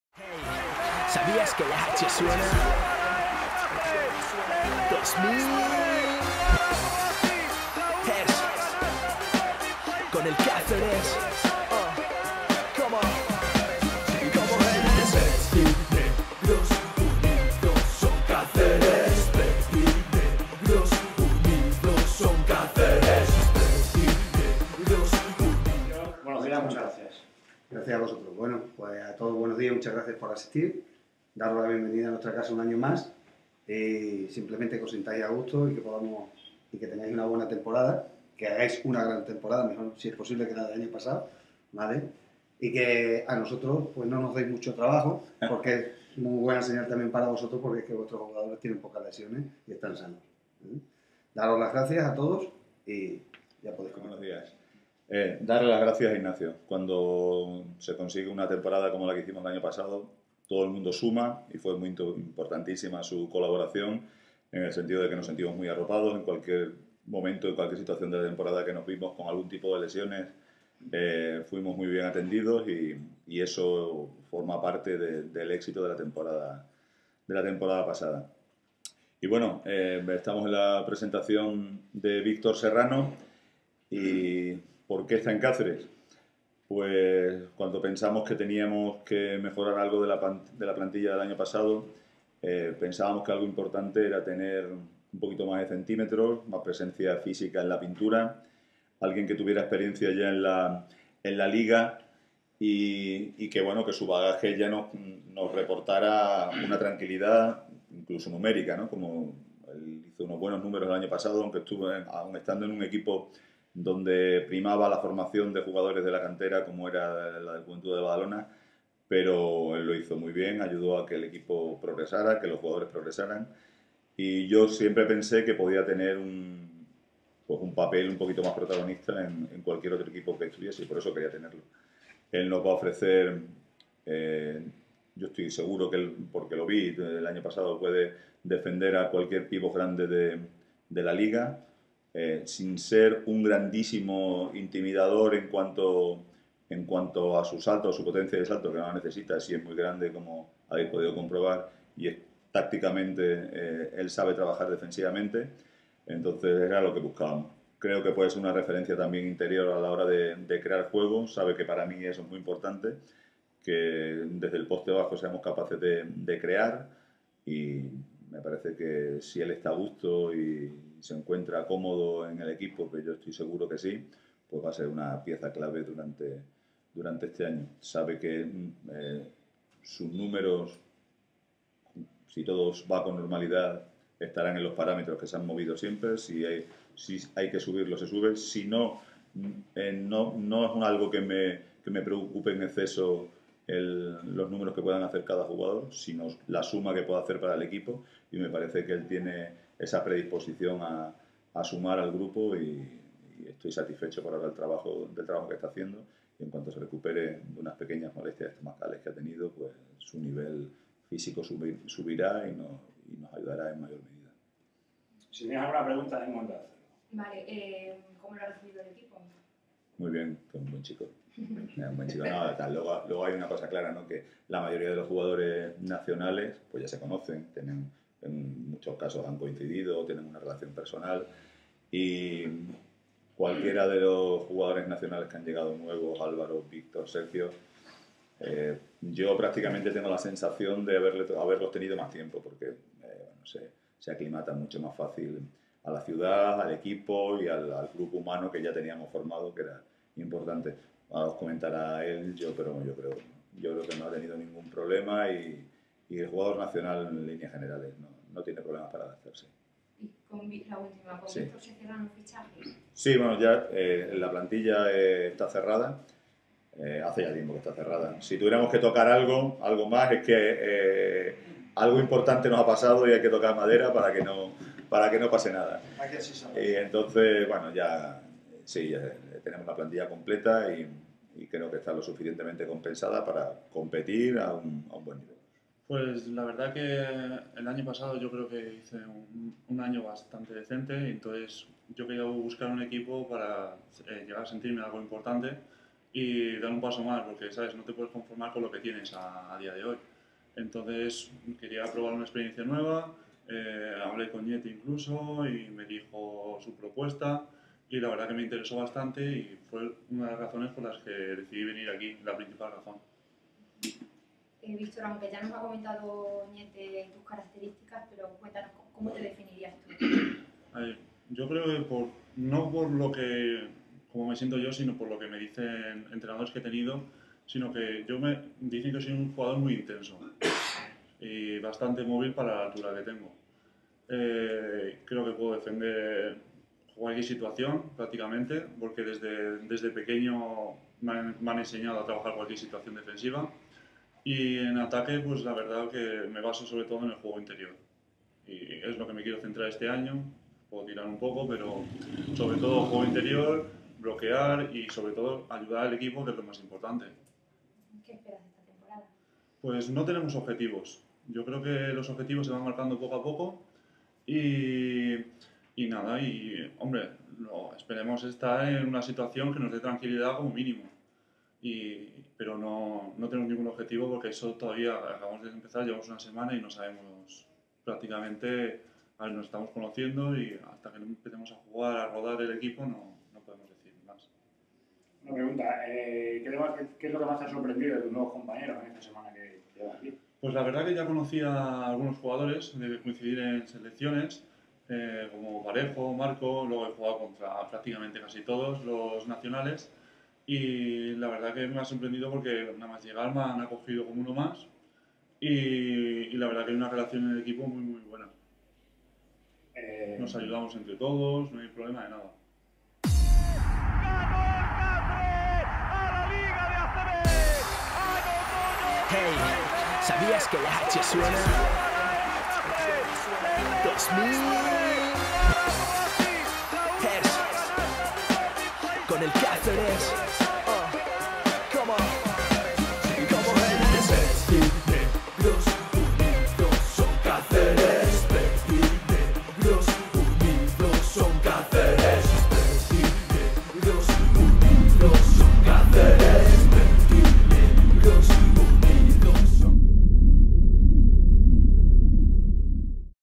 t1516presn vserrano asisa
Caceresbasket 2015/16 - El Cáceres Patrimonio de la Humanidad y ASISA formalizan la firma del acuerdo de renovación de la colaboración que une a ambas entidades con ASISA como proveedor médico oficial verdinegro. Ignacio Barros y José Manuel Sánchez, director gerente de ASISA y presidente del Club, han firmado el acuerdo en la presentación oficial de Víctor Serrano como jugador cacereño. Ñete Bohigas, entrenador del equipo, ha hablado del jugador.